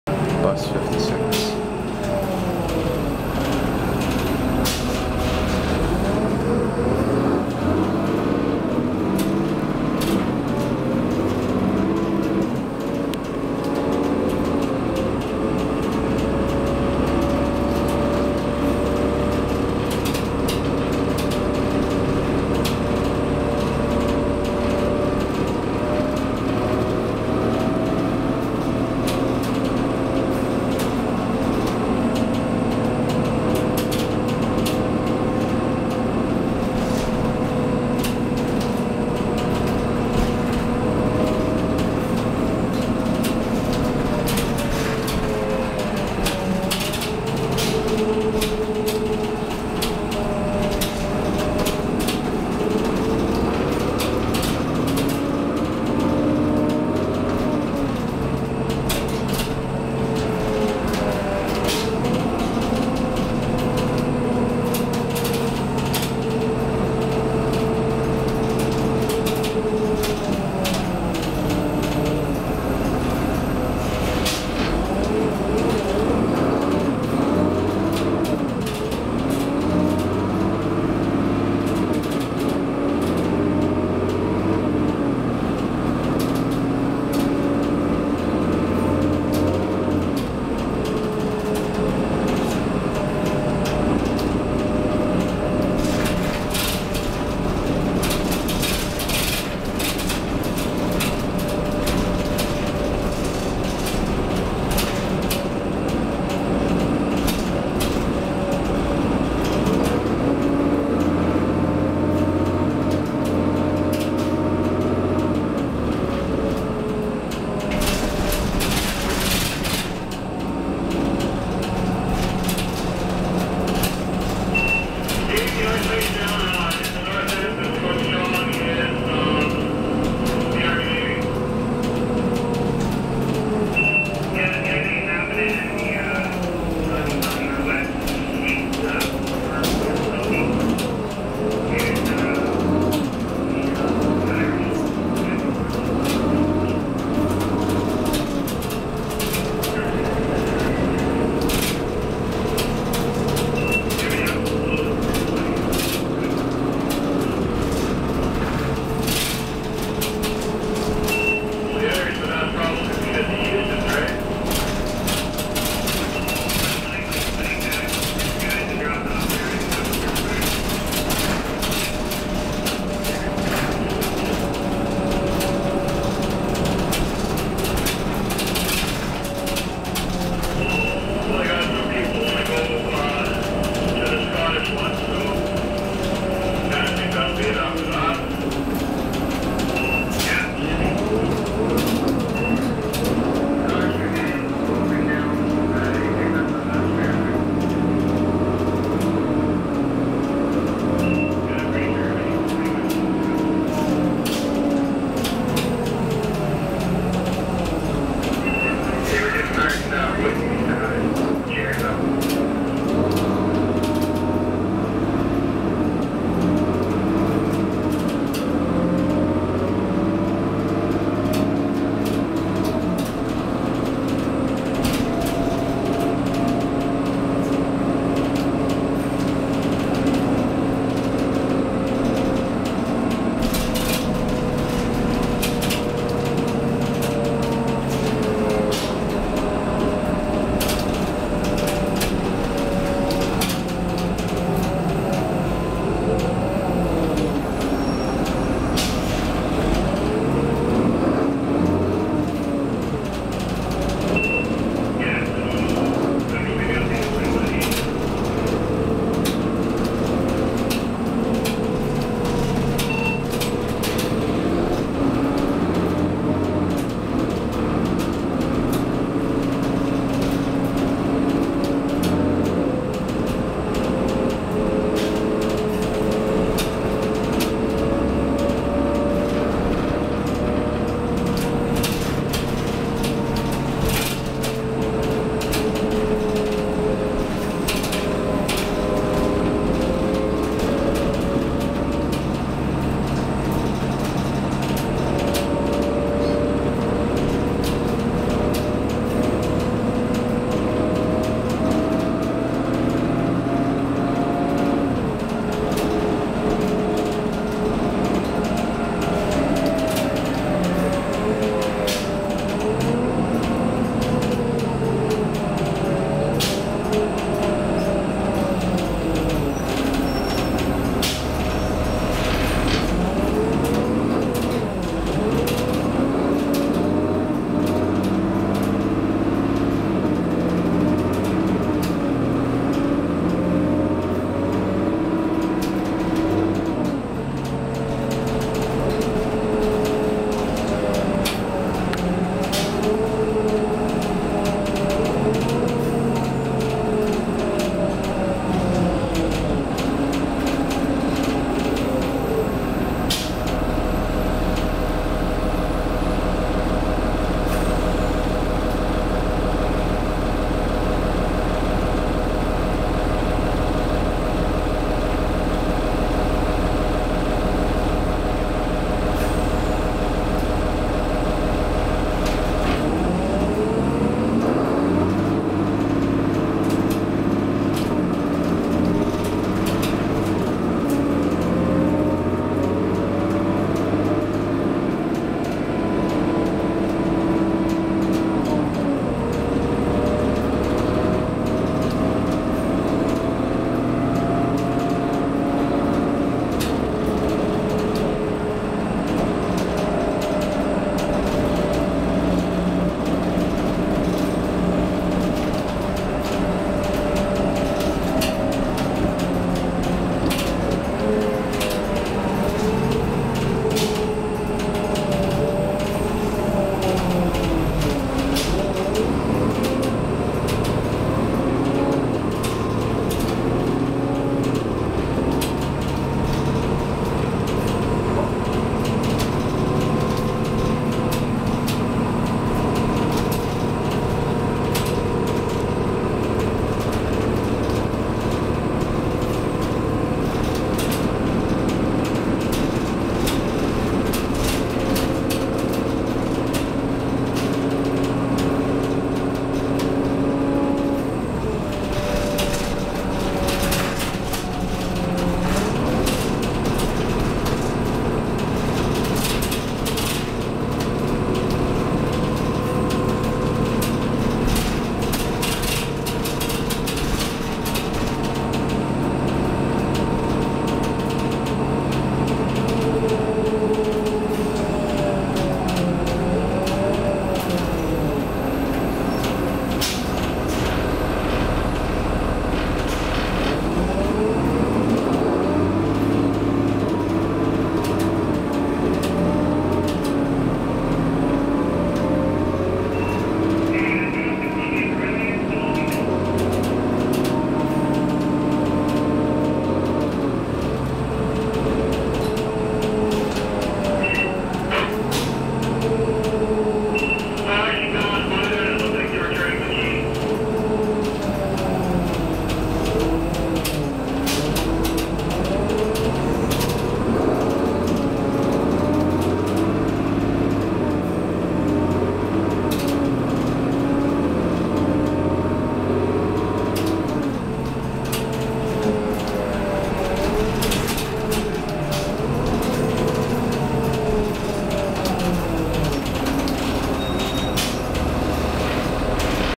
2010 New Flyer D35LFR (Brandon Transit 56)
This bus has a Cummins ISL engine and a Allison B400R transmission.